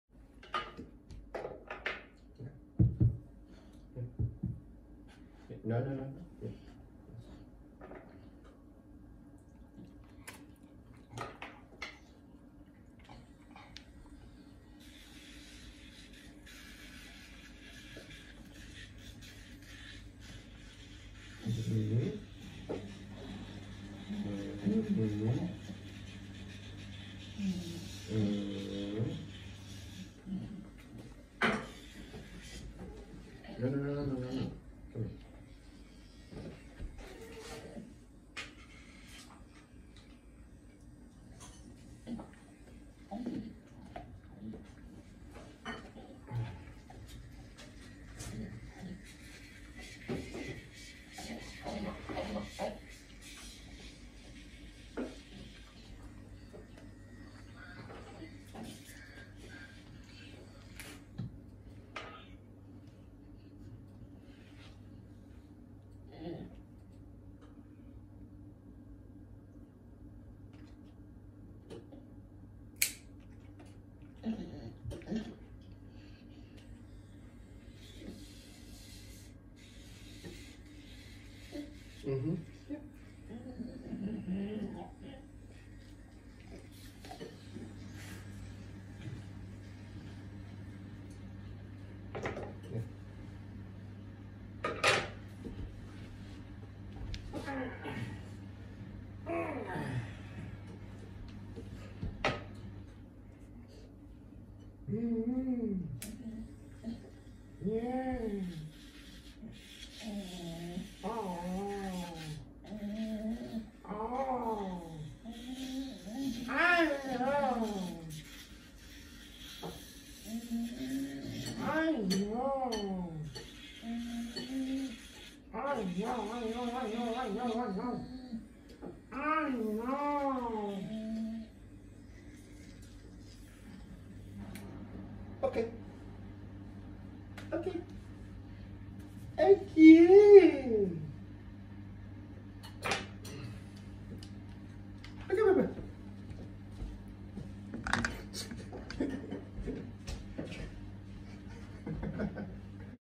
Easy nail care
If your dog is weary about nail care, here are some tips:
1 - Try replacing the cutter for an electric trimmer. It is far less invasive and less painful as well;
2 - Trim nails after walk or long exercise. It will help both dog and human to be relaxed.
3 - treats are always welcome. In this case, something that the dog will be chewing for longer is more recommended.
4 - Make it playful. Make it fun.  When cleaning, most humans treat their dogs as if they are being punished. Nobody likes that, specially when there is some physical discomfort involved.
When you make this or any other grooming procedure playful, the dog will understand that any physical discomfort is mere part of the process and never intentional.